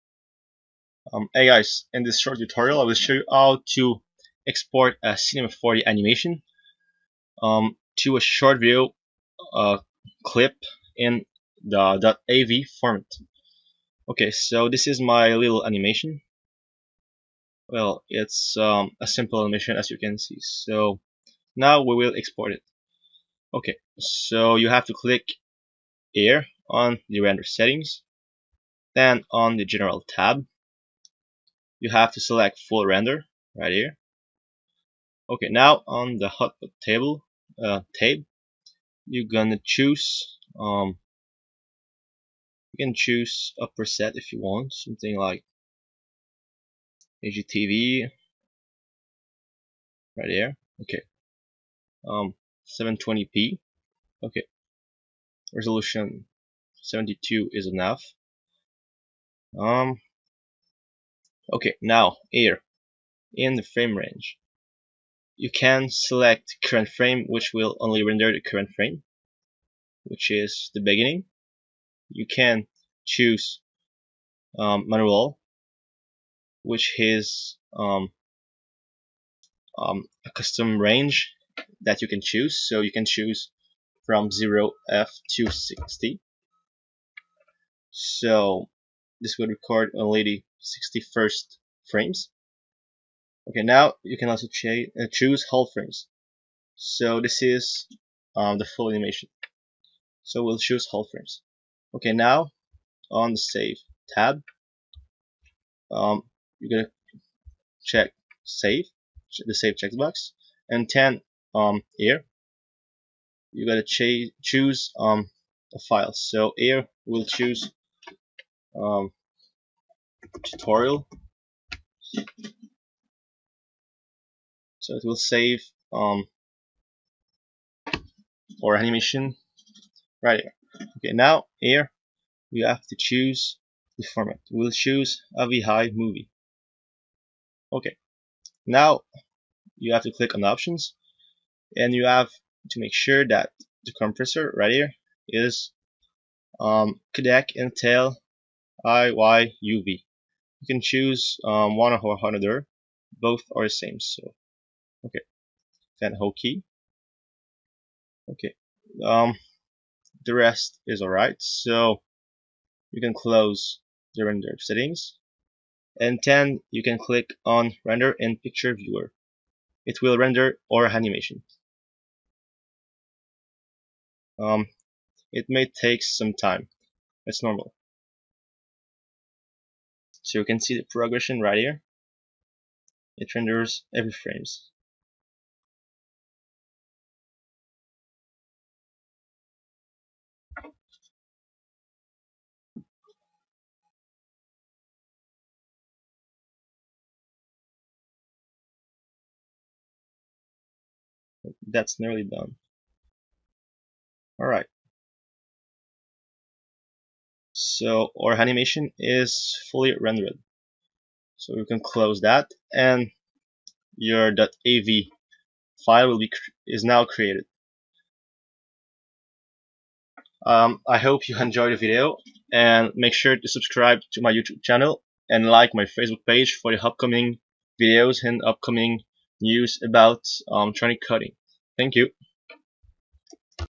[Cinema 4D] Export animation as .AVI video clip
Hey guys! In this short tutorial I'll show you how to export a Cinema 4D animation as a .AVI video clip.